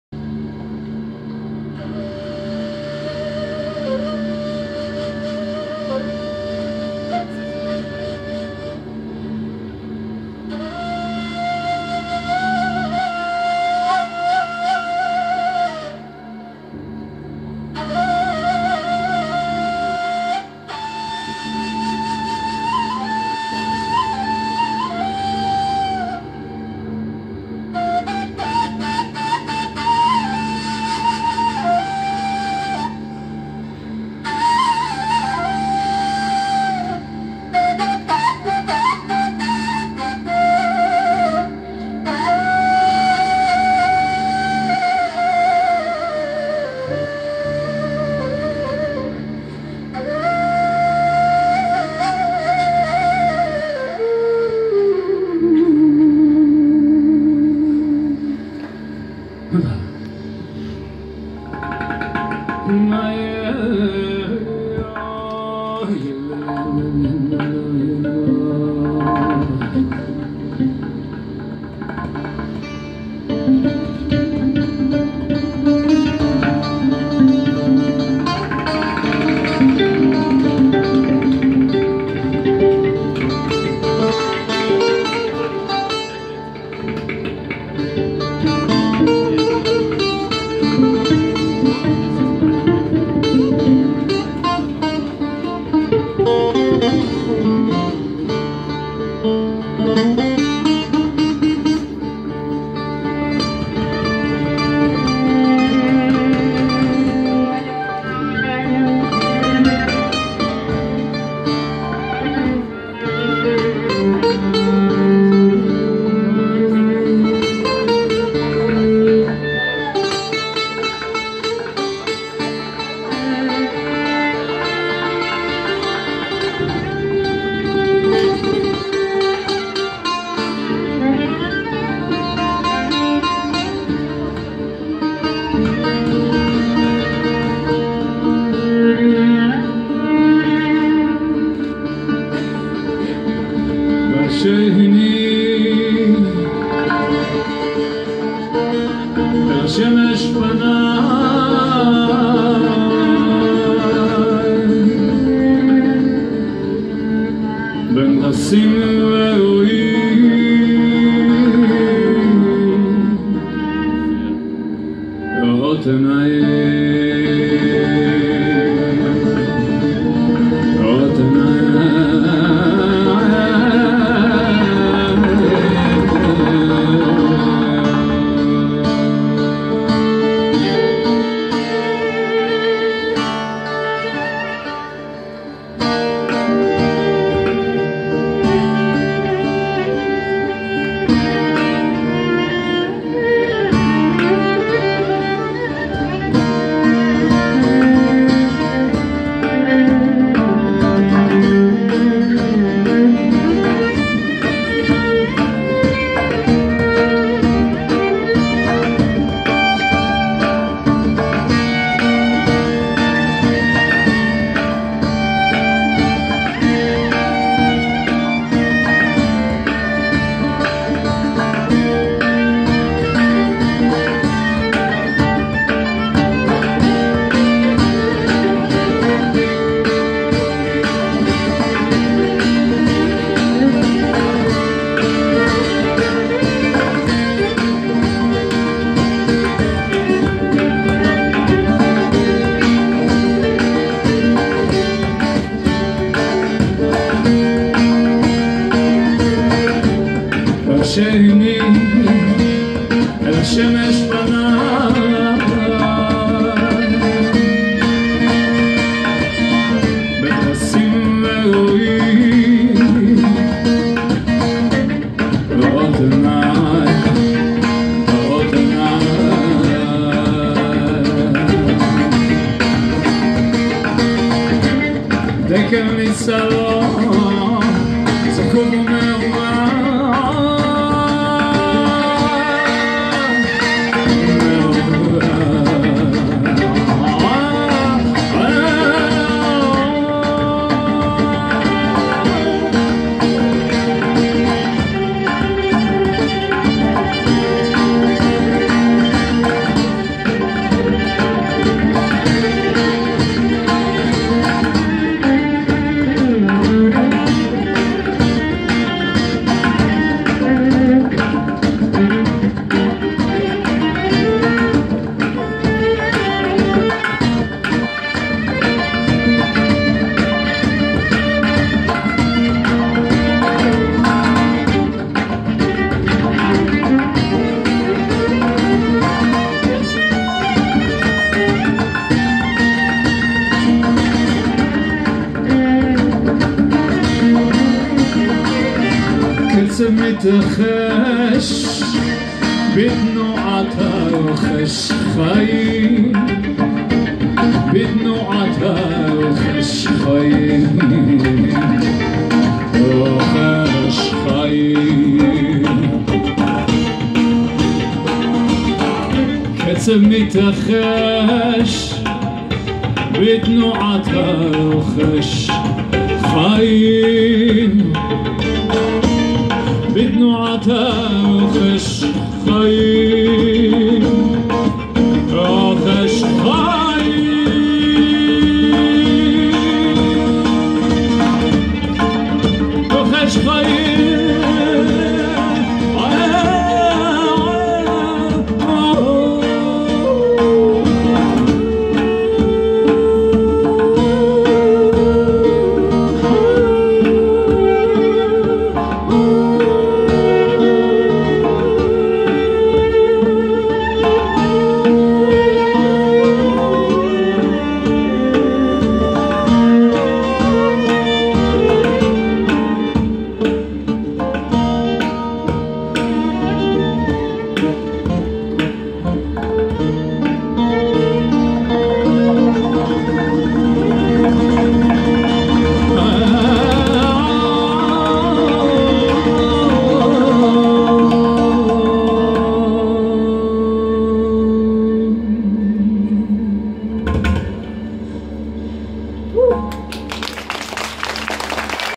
Davide Swarup, Avi Adir & Julien Moretto feat. Veronika Nadtochiy (Improvisation)
Improvisation, modern belly dance. TranSsibi Tour 2015